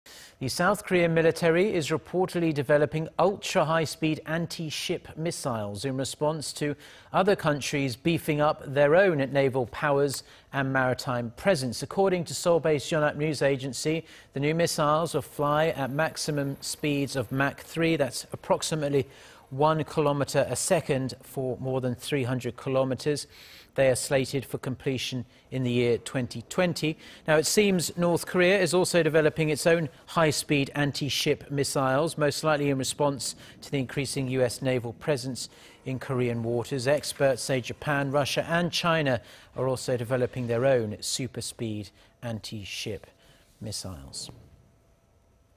S. Korea reportedly developing high-speed anti-ship missiles
군 초음속 대함미사일 개발..2020년 실전배치 목표
The South Korean military is reportedly developing ultra high-speed anti-ship missiles, in response to other countries beefing up their naval powers and maritime presence.
According to Yonhap News Agency, the new missiles will fly at maximum speeds of Mach 3, or approximately one-kilometer a second, for more than 300 kilometers.
They're slated for completion in 2020.
It seems North Korea is also developing its own high-speed anti-ship missiles, most likely in response to the increasing U.S. naval presence in Korean waters.
Experts say Japan, Russia and China are also developing their own super-speed anti-ship missiles.